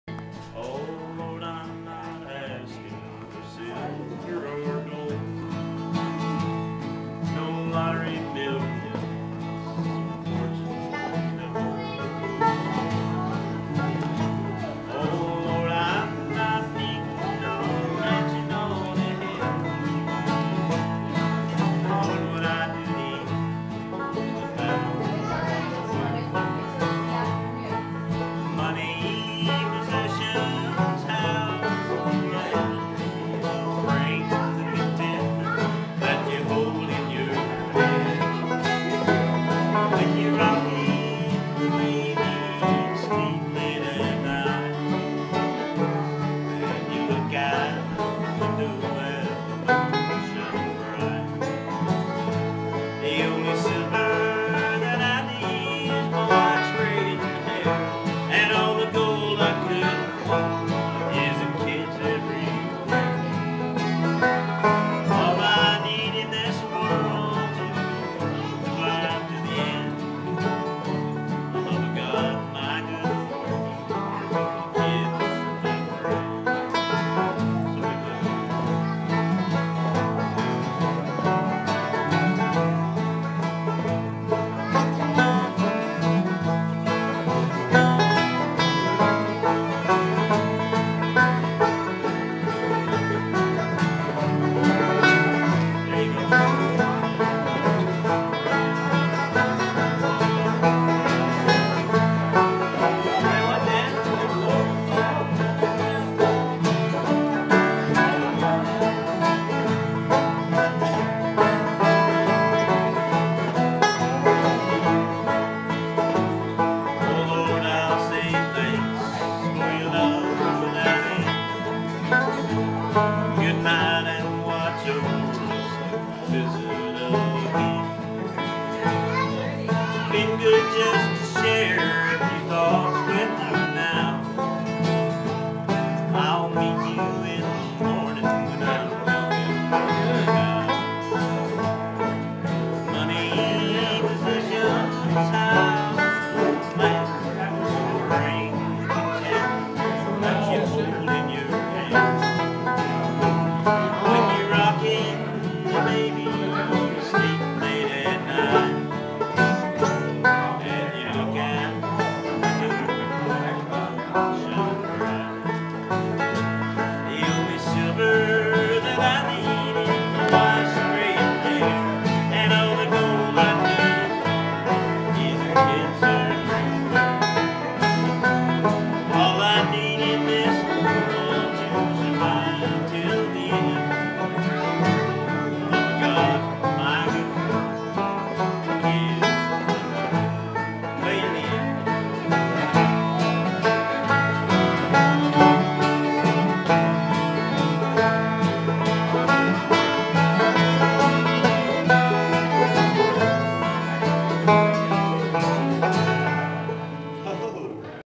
O'Neel '09 Music Jam - Bill Snocker
Muscians from around the state gather each year to celebrate the O'Neels anniversary numbering over 50 by the end of the weekend.